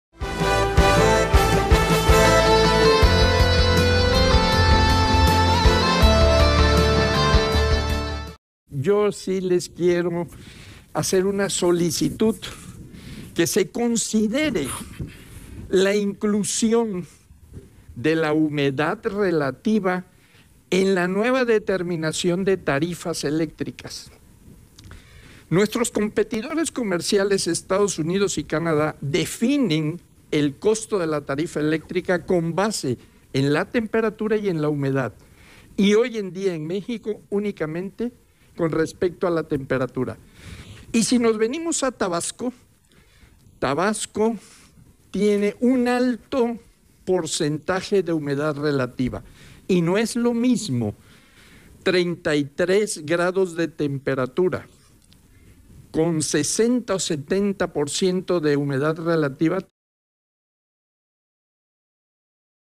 Plantea especialista reubicar estaciones de medición térmica y rediseñar escalones tarifarios
Pide el consultor Adolfo Emilio Bullé a legisladores consideren en reforma eléctrica incluir el factor humedad relativa en la determinación de tarifas, reubicar estaciones de medición térmica y rediseñar escalones tarifarios.